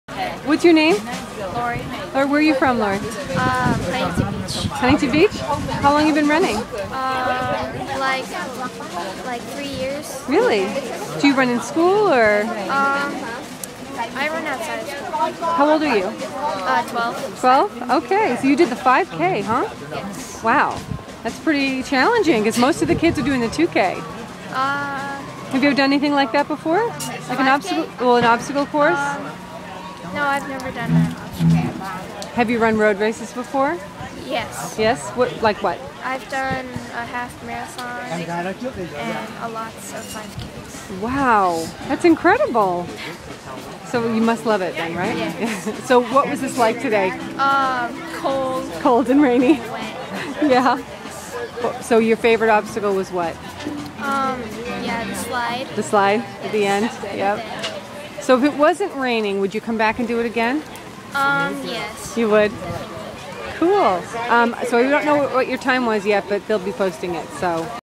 Surf City Beach Derby
Lori, 12-year-old runner from Huntington Beach. She rocks!